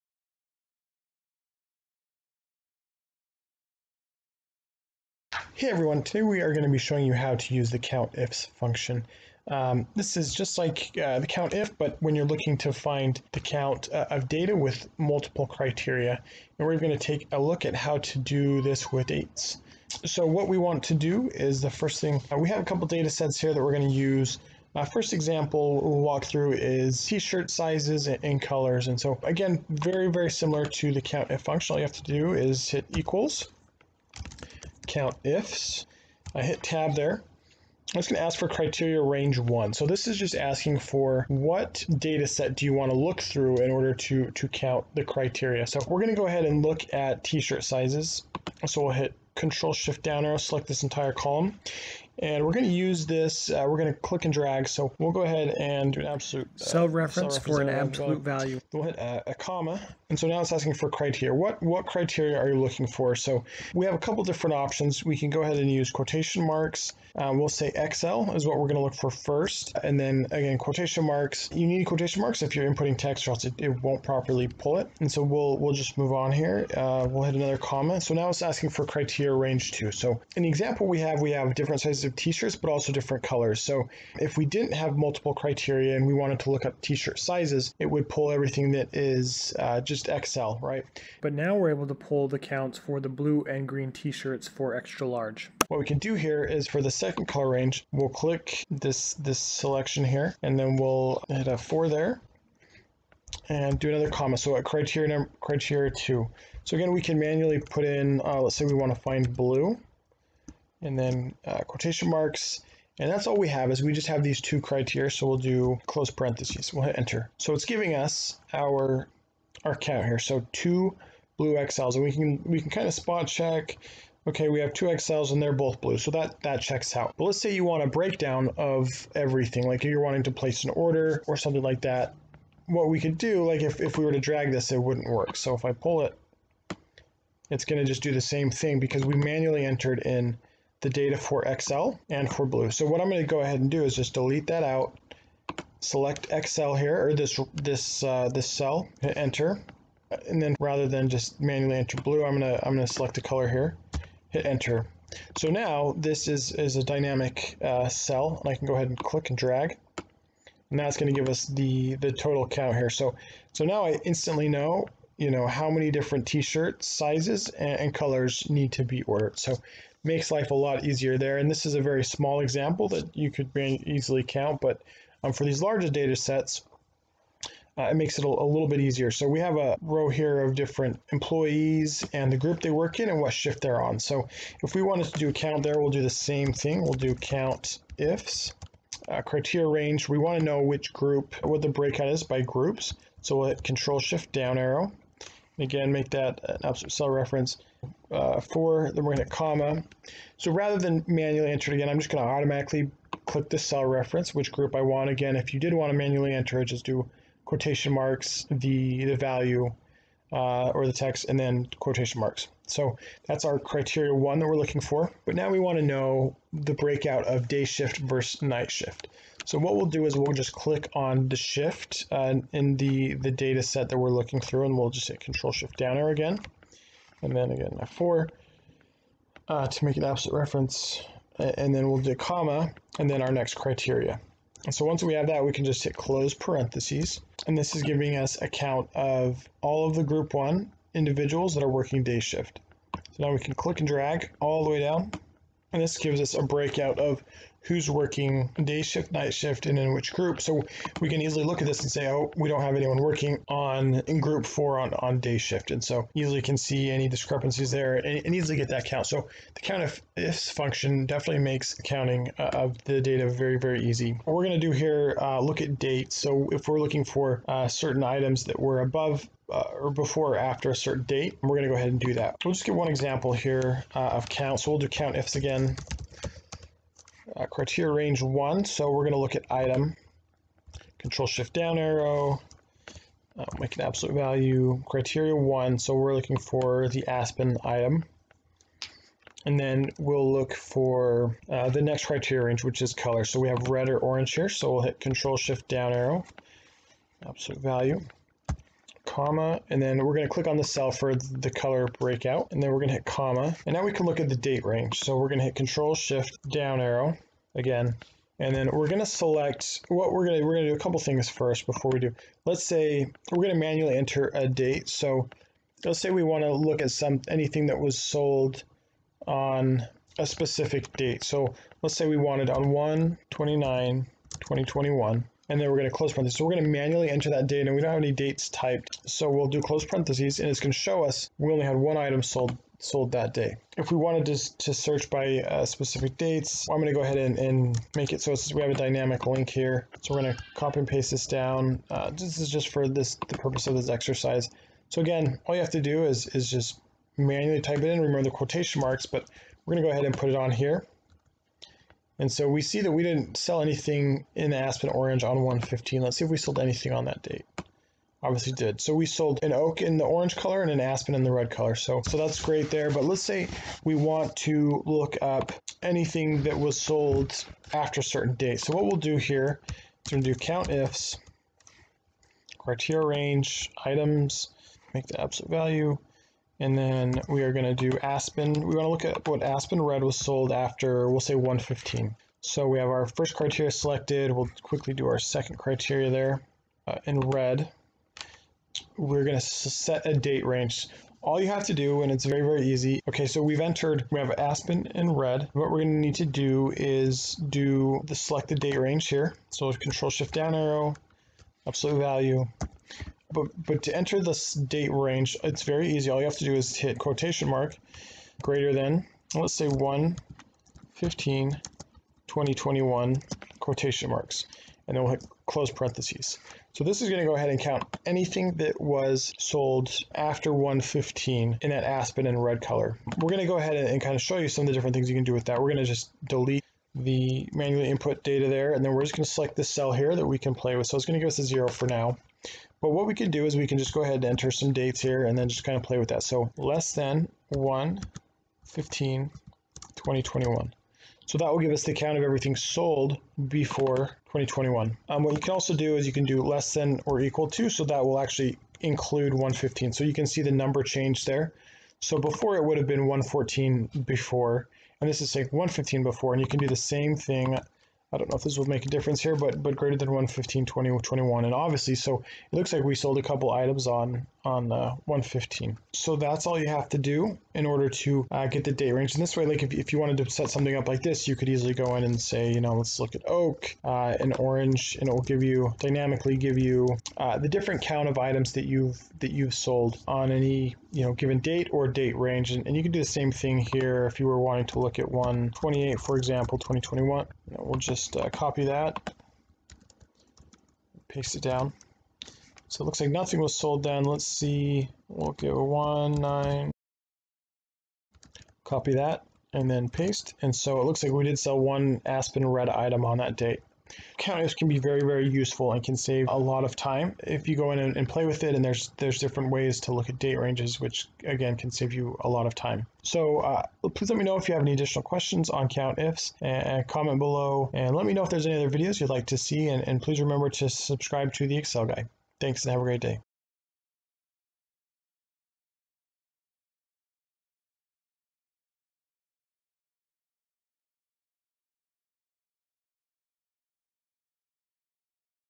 How to use the COUNTIFS function formula in Excel video
I will show you how I use the countifs function formula in Excel.